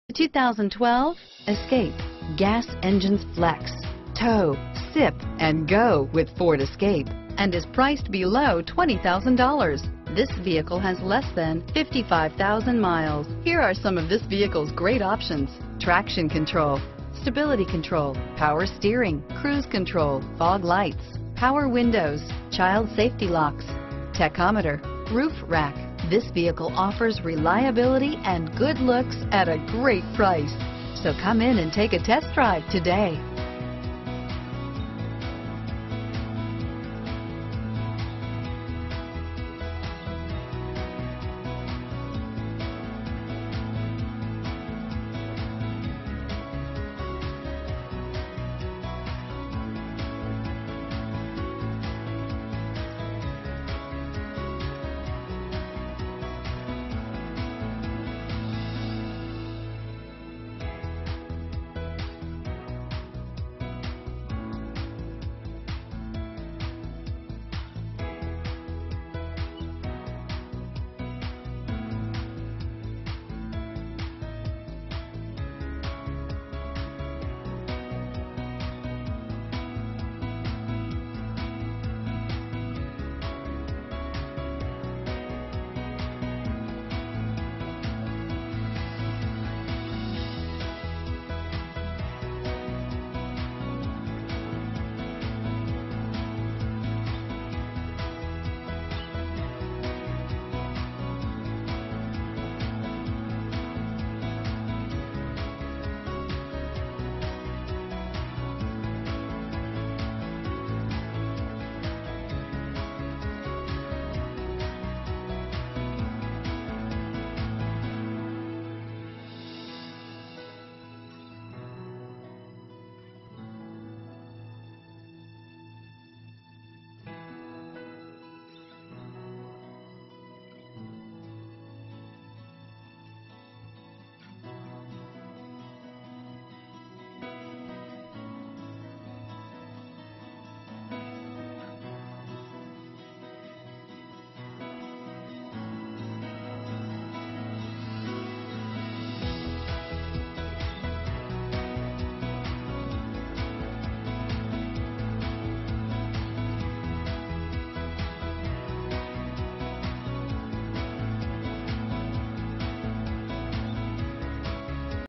2012 Ford Escape Columbus, Delaware, Westerville, Gahanna, New Albany, OH B40112
2012 Ford Escape

Krieger Ford
1800 Morse Road

Our 2012 Ford Escape offers five-passenger seating, high ground clearance, and a high seating position that drivers look for in a smaller crossover. Packing an energetic 3.0L Flex Fuel V6 engine and a smooth as silk 6-Speed automatic transmission, this Front Wheel Drive tosses out 240hp while scoring 23mpg highway. Go ahead and load up all the gear. We're headin' out! In terms of technology, comfort, and convenience, Ford has traditionally been on the cutting edge, and our 2012 Escape is no exception. This XLT trim adds such comfort and convenience features as a power driver's seat, keyless entry keypad, multifunction steering wheel, automatic headlights, a rockin' audio system, a sunroof, and much more. Scan our pictures and the long list of features. As for our best comfort feature - SAFETY - AdvanceTrac with Roll Safety Control, a tire pressure monitoring system, and a MyKey to control the speed of your teen driver are a few of the great features on board.